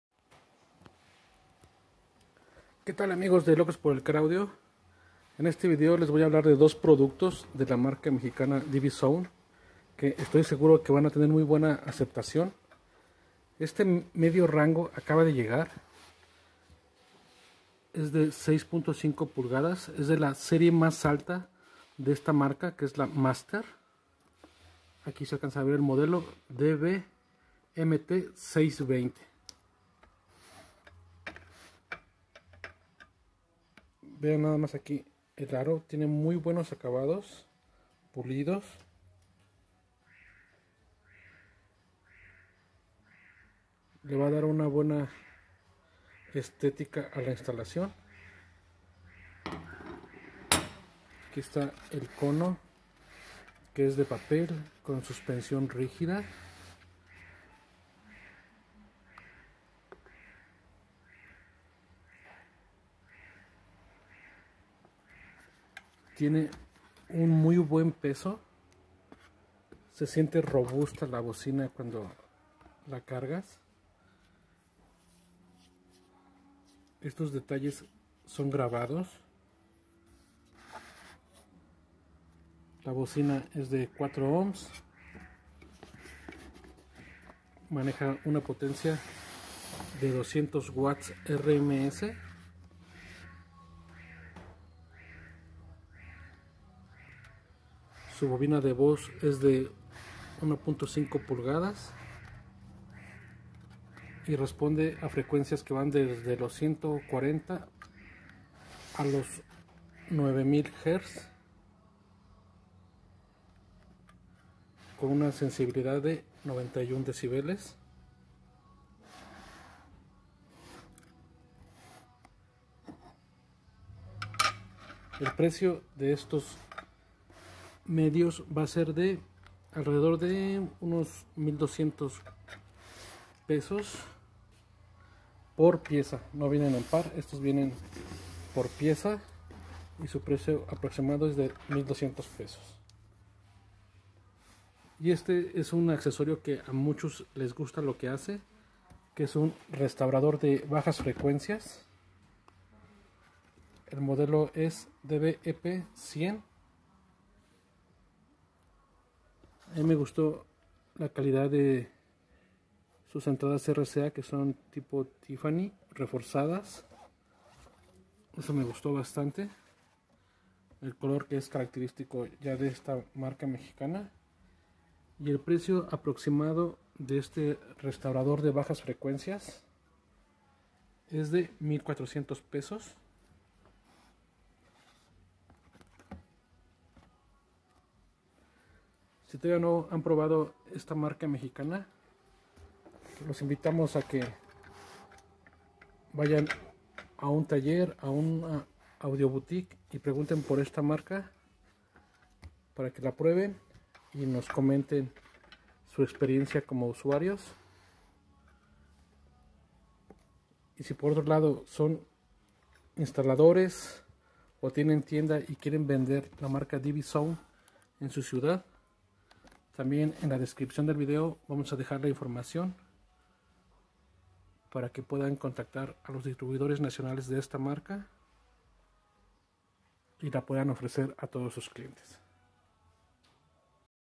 Medios Rangos y Restaurador de Bajas Frecuencias DB Sound
Ahora les presentamos dos productos de la nueva marca mexicana DB Sound.
Si ya eres el propietario de uno de estos equipos, comparte con nosotros tu experiencia de uso.
Para más detalles pueden visitar la página oficial de la marca